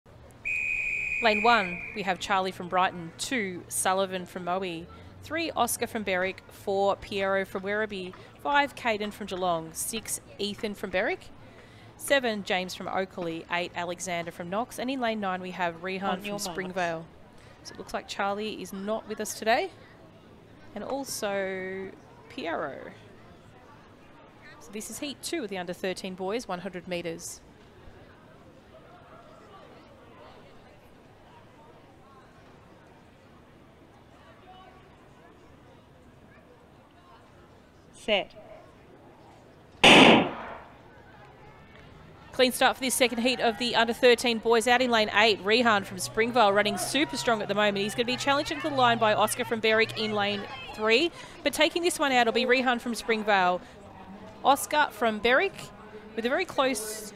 U13 Boys 100m Heat 2: 2022-23 State Combined Events Championships - Saturday
SHORT DESCRIPTION: U13 Boys 100m Heat 2: 2022-23 State Combined Events Championships - Saturday